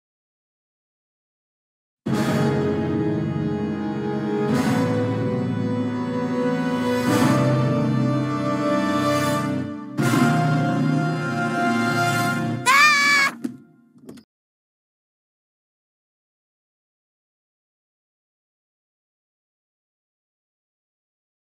Xe điện Trung Quốc mua mấy tháng Super ME GT - rỉ sét nhanh
Hi vọng mẫu mới sẽ tốt hơn